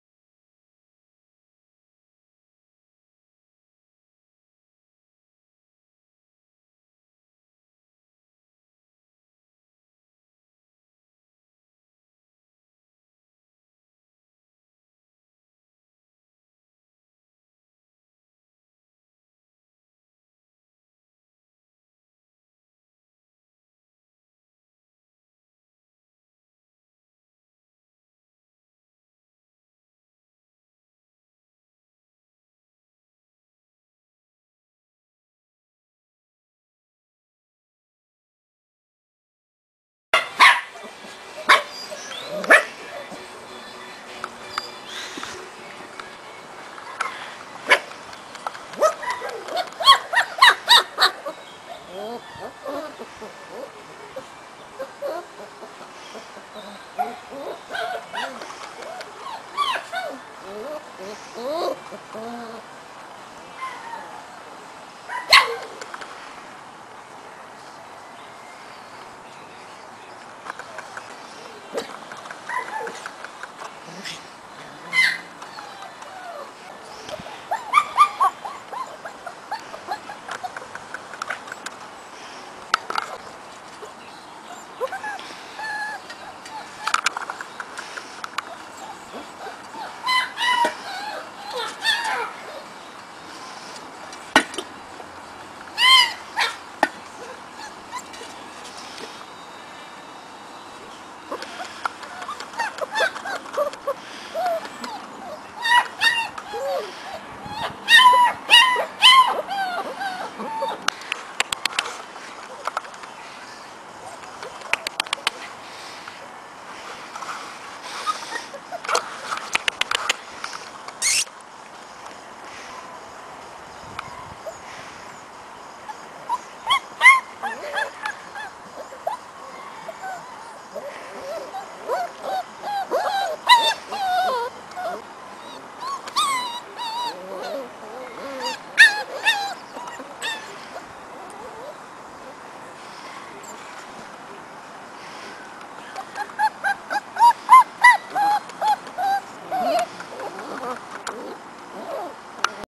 tumi grem pups
Pups @ 4 weeks old. First time out and first time wearing a collar.

Tumi von der Schiffslache x Grem du Triangle Magique